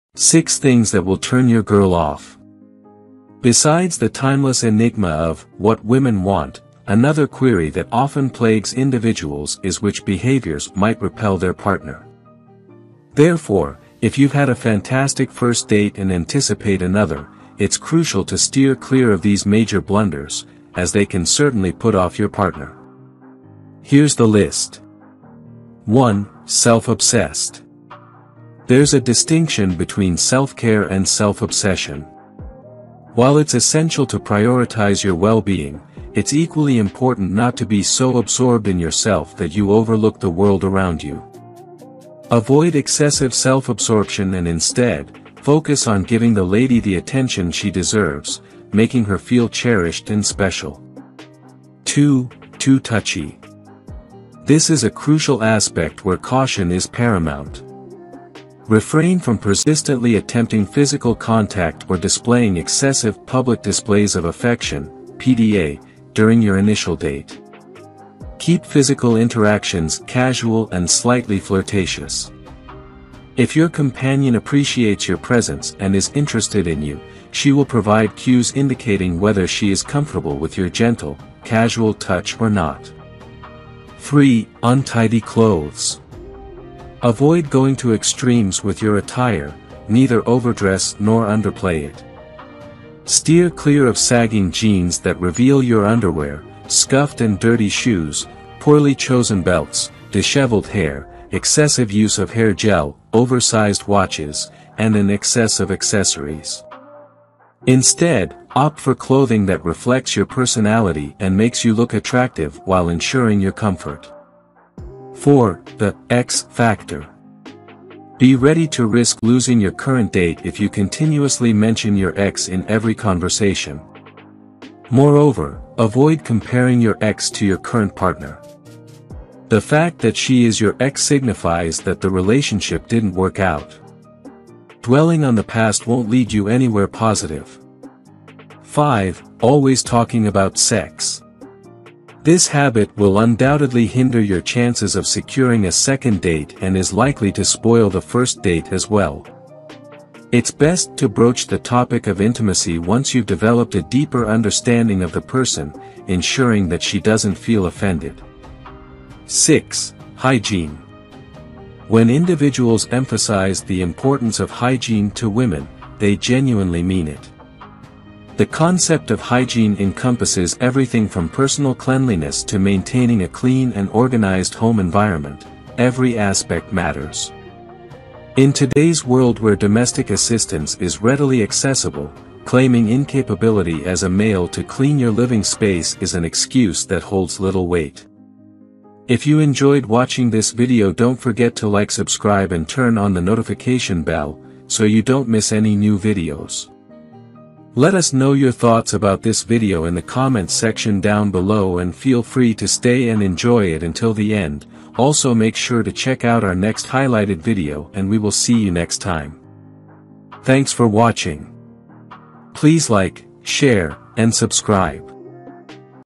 6 Things That Will Turn Your Girl Off | Psychology Facts About Human Behaviour | Awesome Facts
6 Things That Will Turn Your Girl Off | Psychology Facts About Human Behaviour | Awesome Facts
Besides the timeless enigma of "what women want," another query that often plagues individuals is which behaviors might repel their partner. Therefore, if you've had a fantastic first date and anticipate another, it's crucial to steer clear of these major blunders, as they can certainly put off your partner. Here's the list:
1. Self obsessed:
2. Too touchy:
3. Untidy clothes:
4. The 'ex' factor:
5. Always talking about s-x:
6. Hygiene: